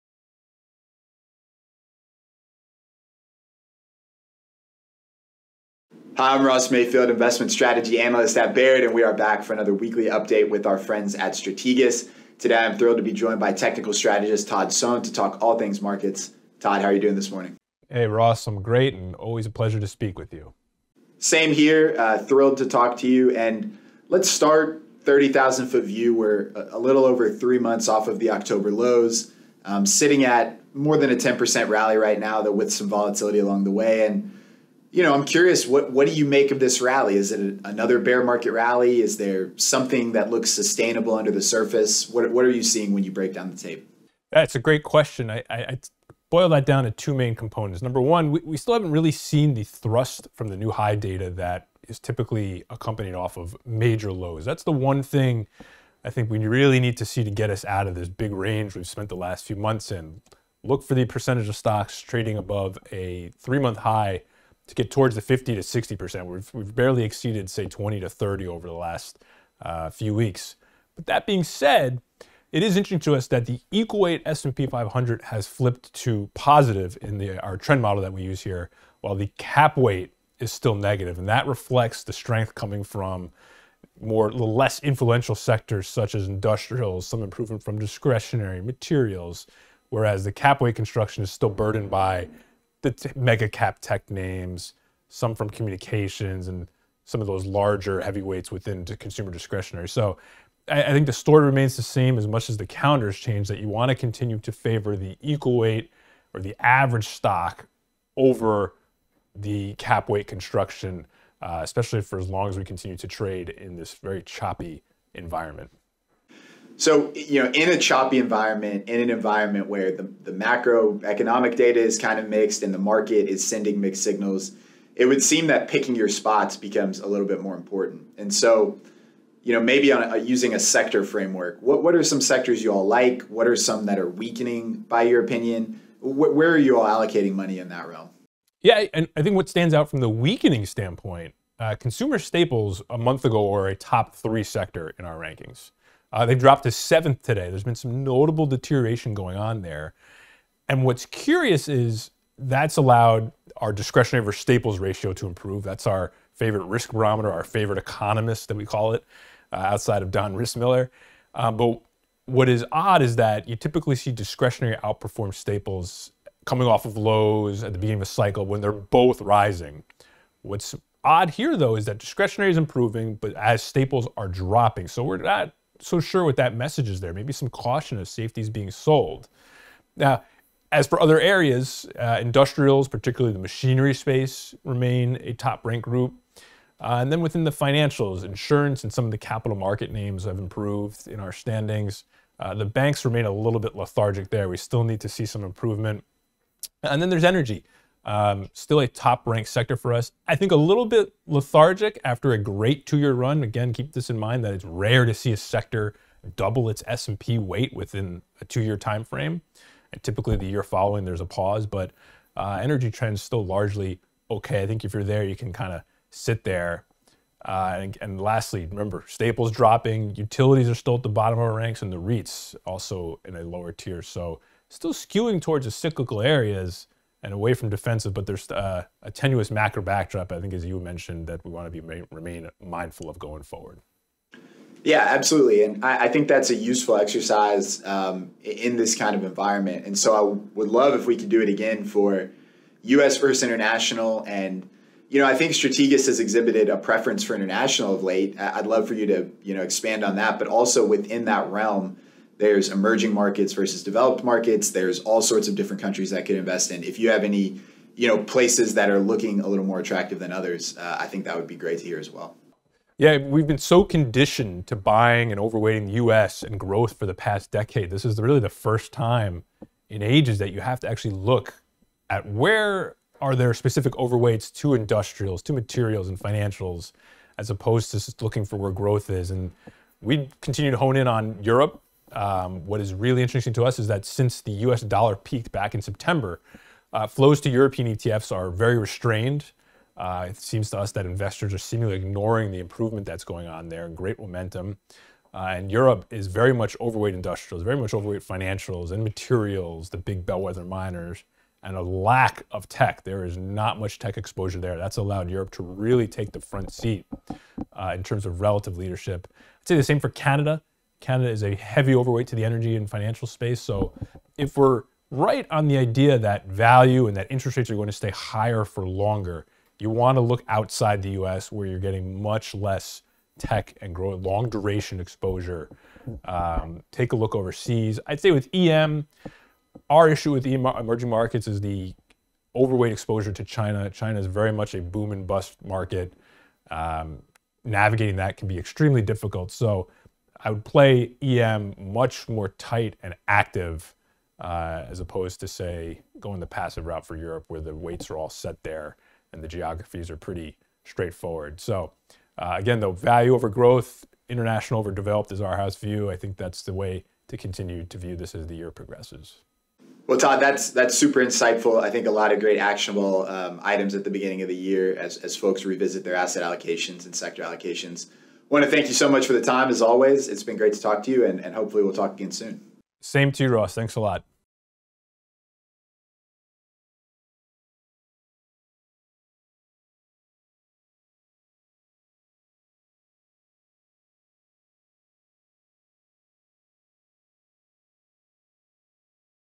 Market Strategy Weekly - January 20, 2023
Strategas Technical Strategist Todd Sohn walks us through the stock market landscape and provides some areas of opportunity and weakness. Todd discuss the U.S. stock outlook, which sectors are outperforming, and how to break down U.S. vs. International.

For more thoughts on how the markets intersect with monetary policy, geopolitical tensions, inflation and other macroeconomic factors,